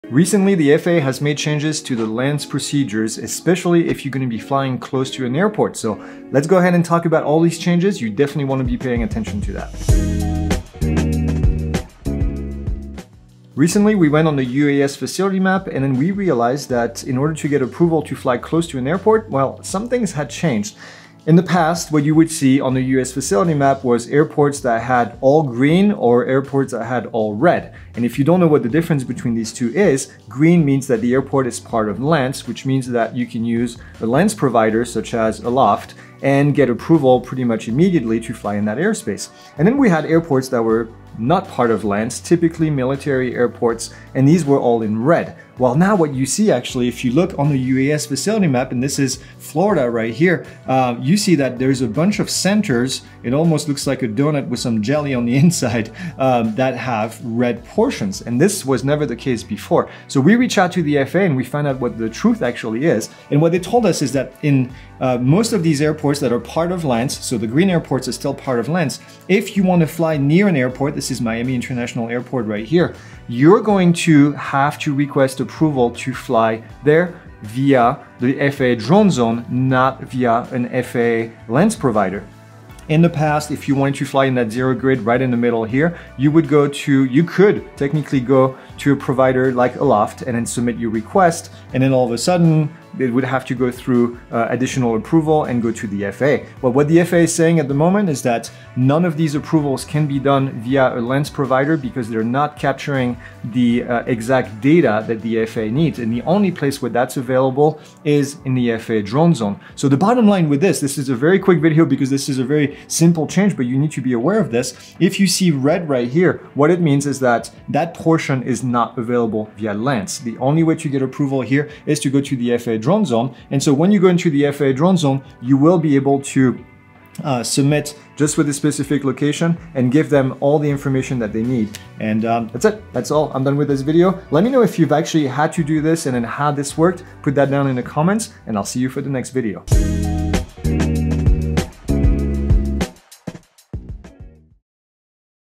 New UAS Facility Maps - Changes to LAANC Authorization in Zero Grids at Airports
The FAA recently changed the UAS facility maps. Certain grids inside of controlled airspace now require a different procedure to get airspace authorization. LAANC providers can no longer give authorization for flights that occur AT an airport. At this time these are limited to zero grids and are limited to airport property. For authorization, use the FAA Drone Zone.